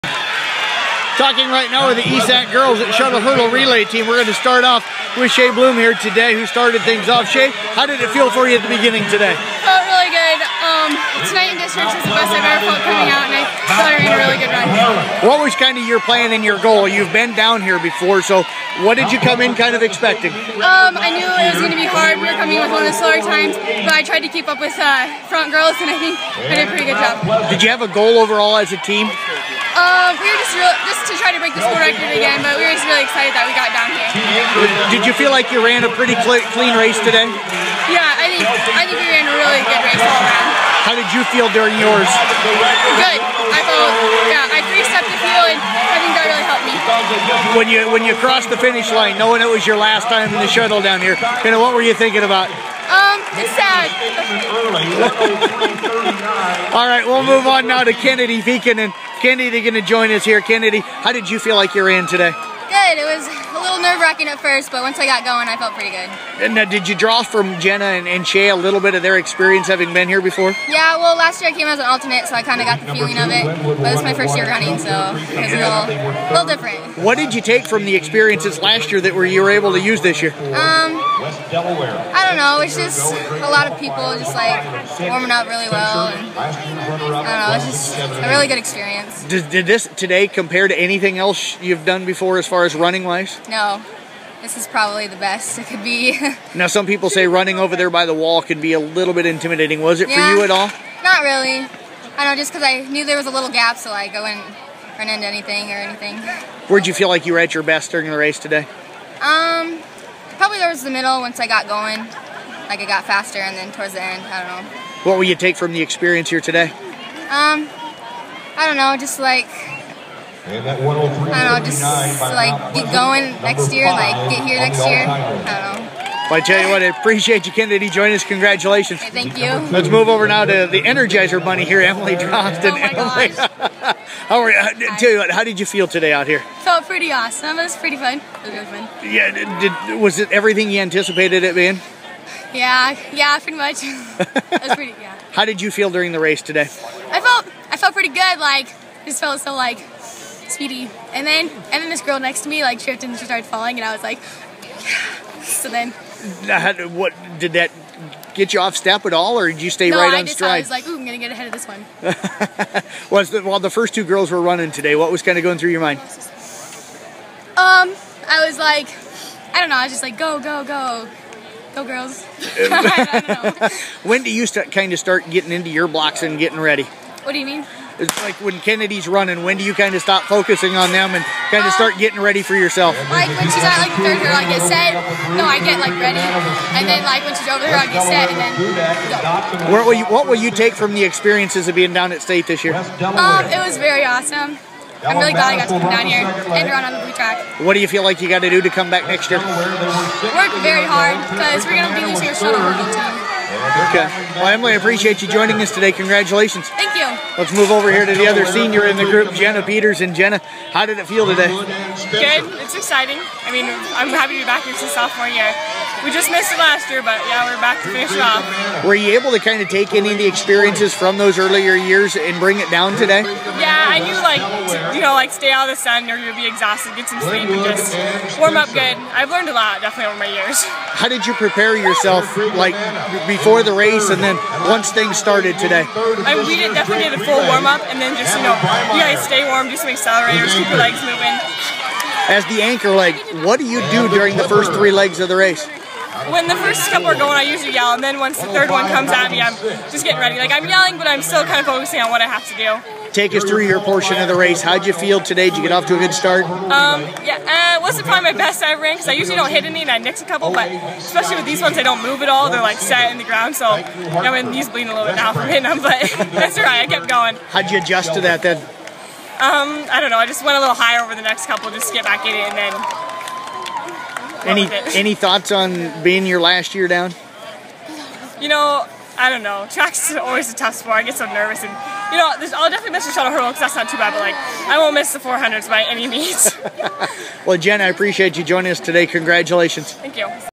East Sac Girls Shuttle Hurdle at State Track
We talked with Shae Blum, Emilee Drost, Kennedy Pieken, Jenna Peters with the East Sac Shuttle Hurdle Team. They finished 3rd in their heat with 1:11.33. And Placed 19th.